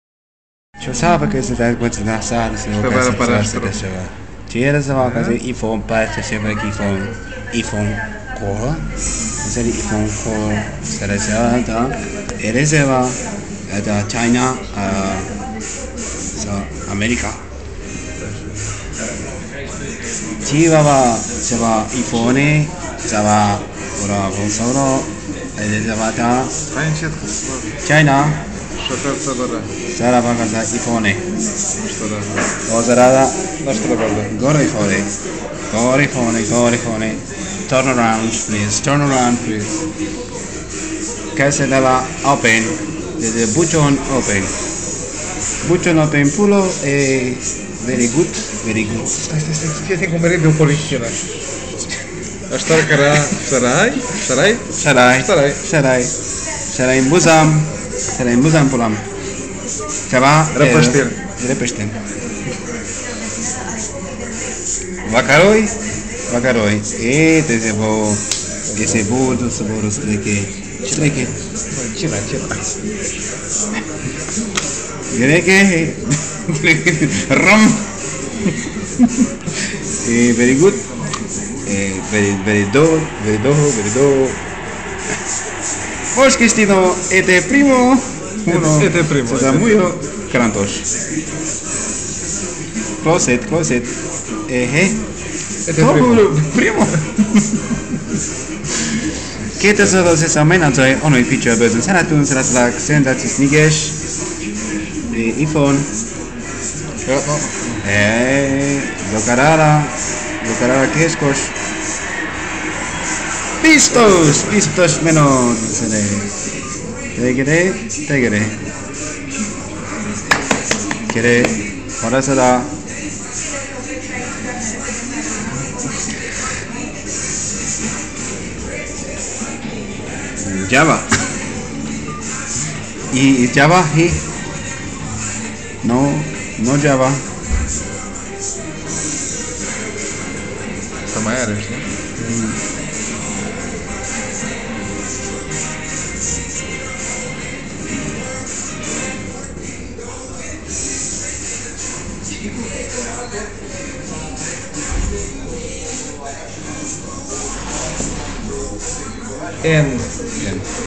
Fake iPhone 4 vs The Real iPhone 4
Replica seems faster than the original. The original is made in China, and the replica is made in California. Funny huh?